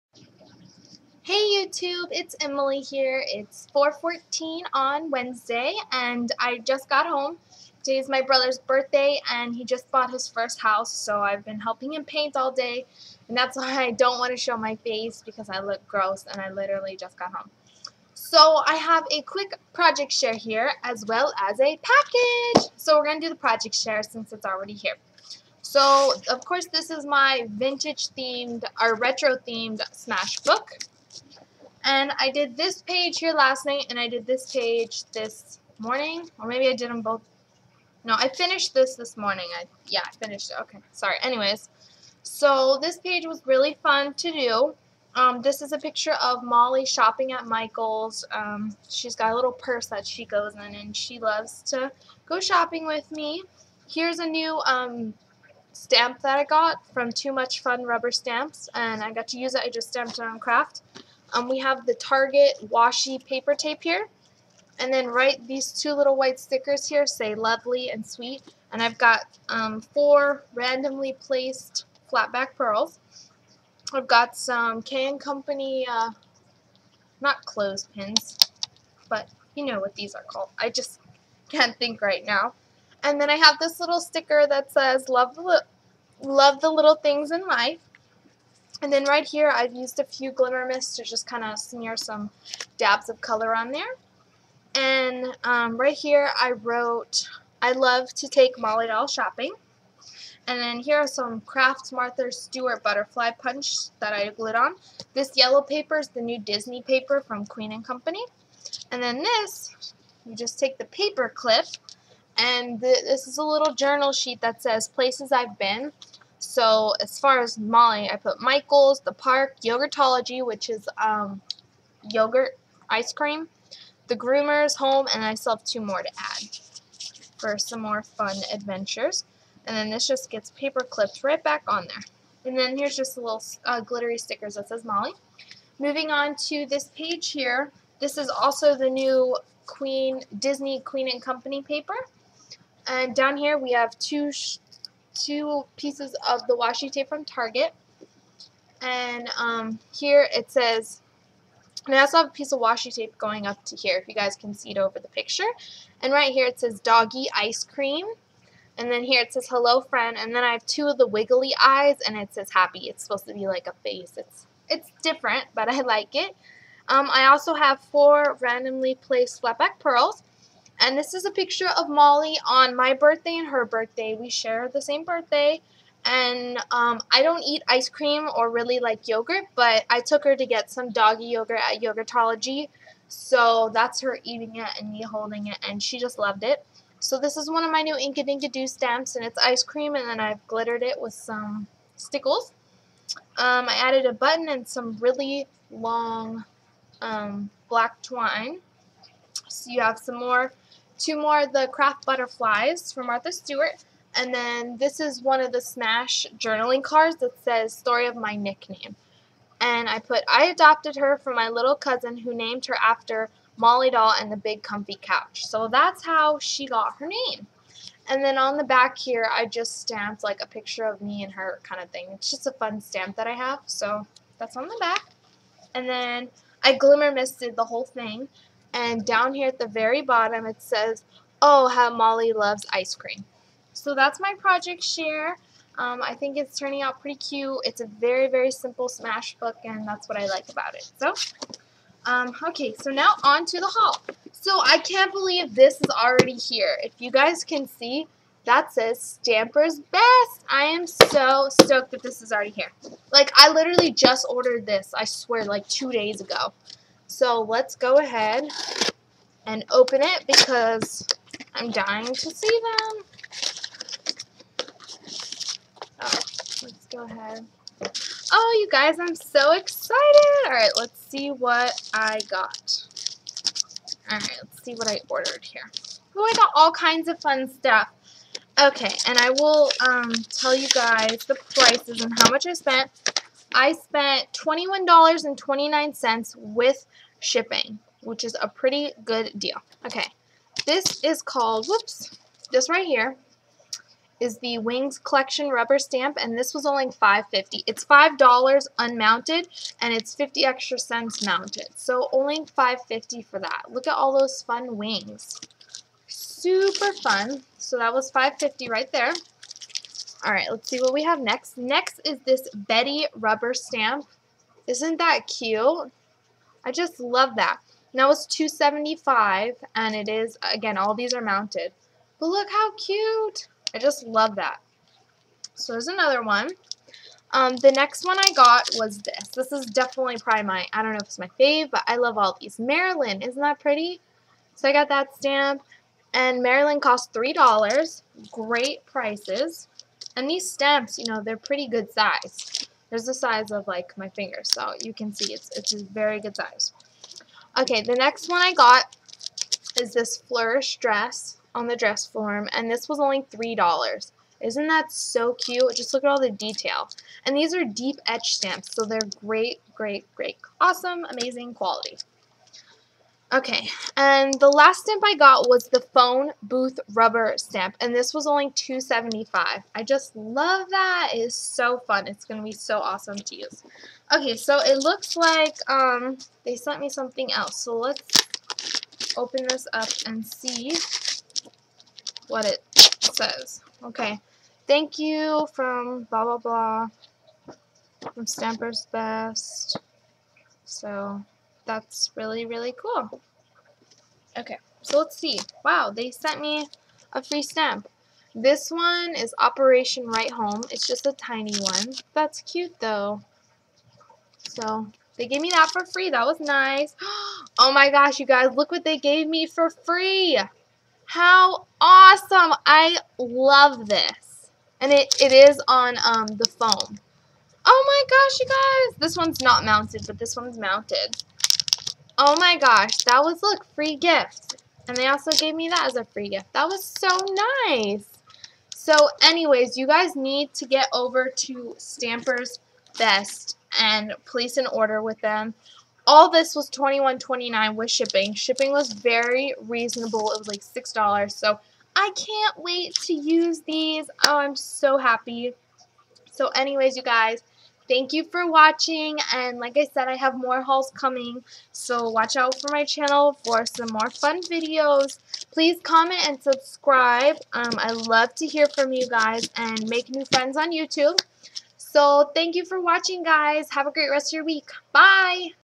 Smash Progress & Stampers best haul
Hope you all like my smash creation and my new stamps!!! I cant wait to use my new stamps on some fun projects-